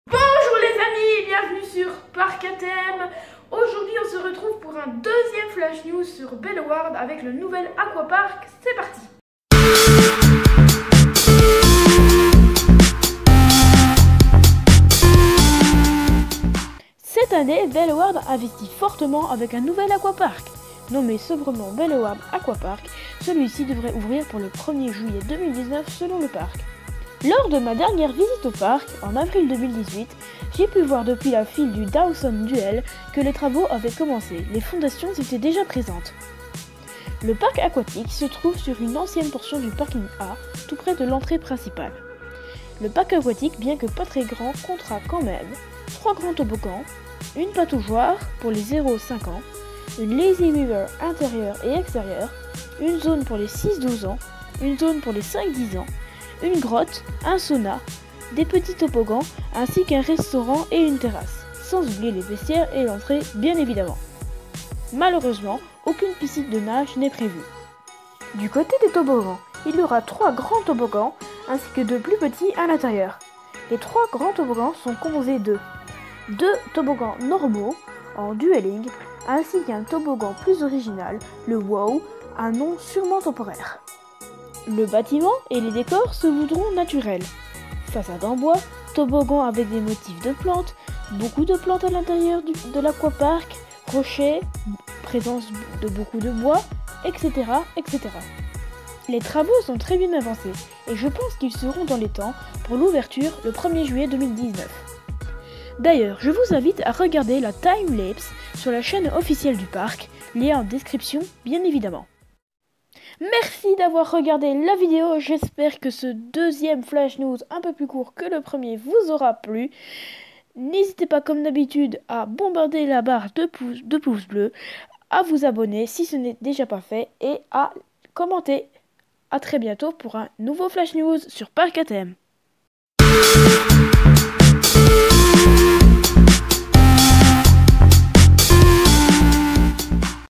📹 FLASH NEWS #2 : MARS 2019 : BELLEWAERDE - AQUAPARK !
Bellewaerde Aquapark est une magnifique aventure pour petits et grands. Découvrez ensemble une oasis de verdure et glissez d'une attraction aquatique à l'autre.